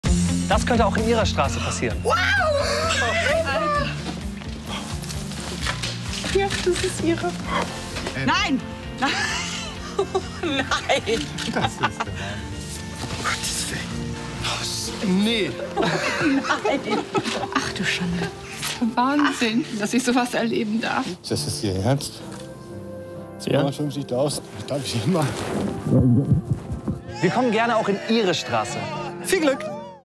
Postcode-Monatsgewinn - der Scheck, der dein Leben verändert
Freudentränen, Sprachlosigkeit, Jubelschreie - jeder Monatsgewinner geht anders mit seinen Emotionen um, wenn die Deutsche Postcode Lotterie mit dem goldenen Umschlag vor der Haustür stehen.

Von Oktober bis März durften sich 25 Teilnehmer aus sechs verschiedenen Städten über einen Besuch der Männer und Frauen in den roten Jacken freuen. Von Bad Nauheim über Hösbach, Köln, Wesel und Loxstedt bis nach Osterrönfeld - überall war die Freude über die Gewinne groß.

Spielteilnahme ab 18 Jahren. Glücksspiel kann süchtig machen. Rat und Hilfe unter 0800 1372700 (kostenlos und anonym).